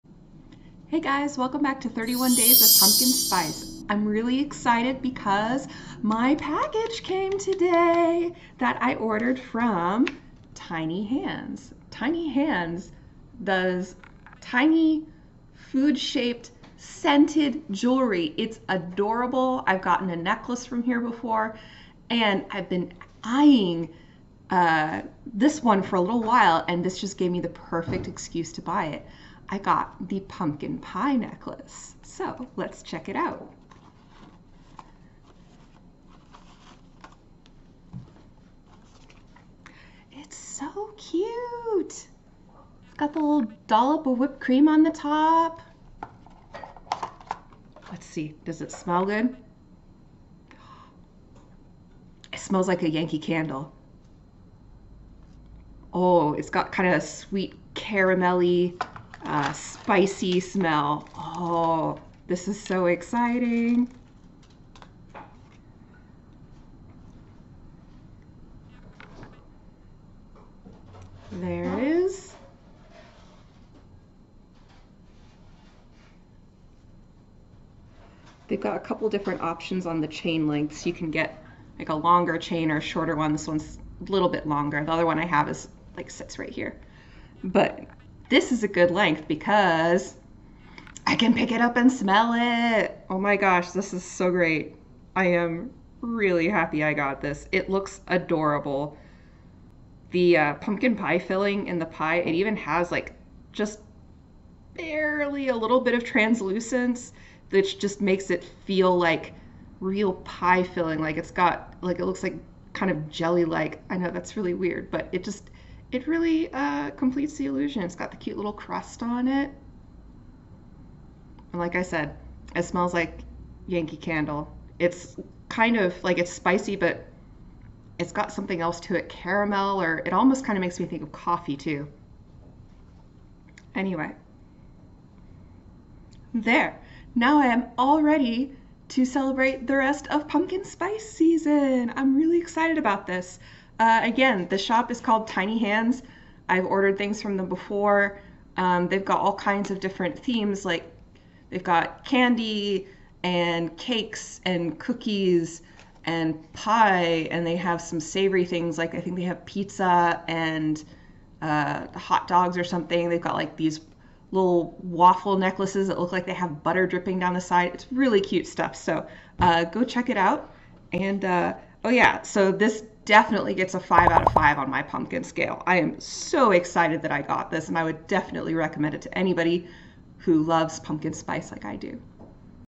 31 Days of Pumpkin Spice | Day 20: Tiny Hands Necklace
I finally got the pumpkin pie necklace I've been eyeing forever! It's tiny, wearable, cute food that smells good, so basically my dream. Go check them out